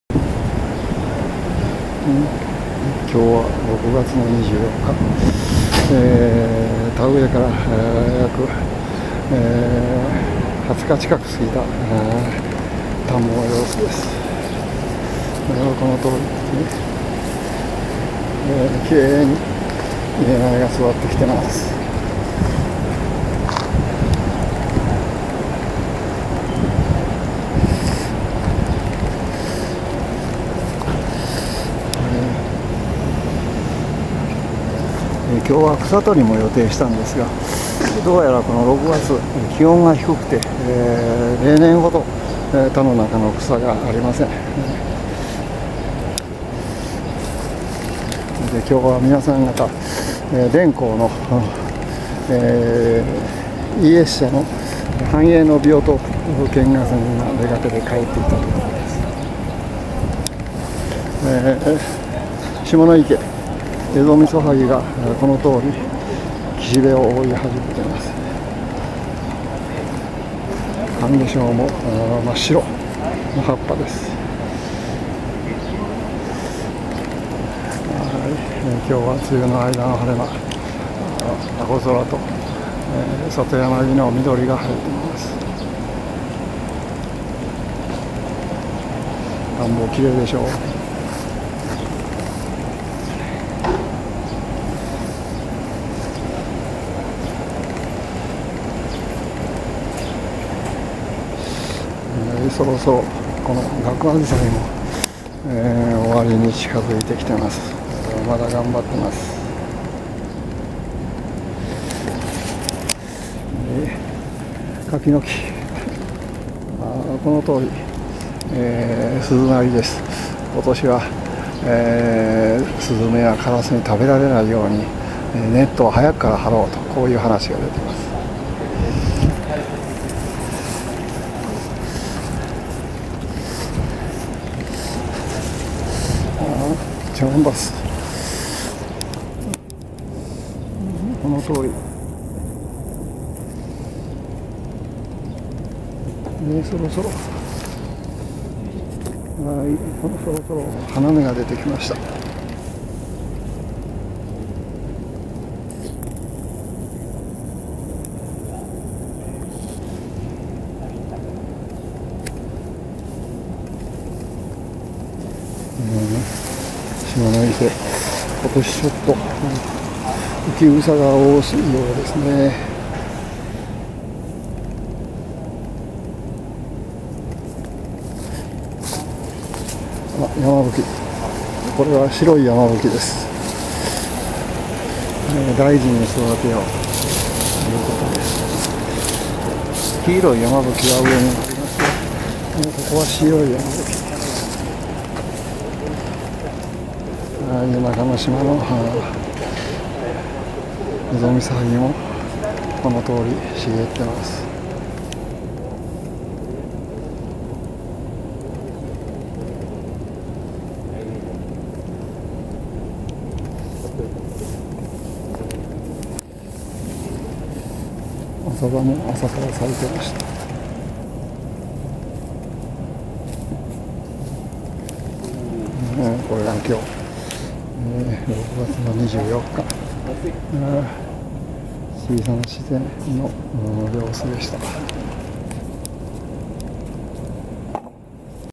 ２０１５ 梅雨の晴れ間 ビオトープ「小さな自然in門真」の様子 The Biotope in Japan
６月、夏至も過ぎ、梅雨の晴れ間が見えたパナソニック エコリレービオトープ 「小さな自然in門真」の様子です。田植えから２０日過ぎ、イネ苗も根をしっかり張ったようです。田の周りで里山木のミドリ濃くなり、小川土手のハンゲッショウは葉っぱ白く染め、岸辺囲むエゾミソハギは紅色の花下記をめぐらし始めました。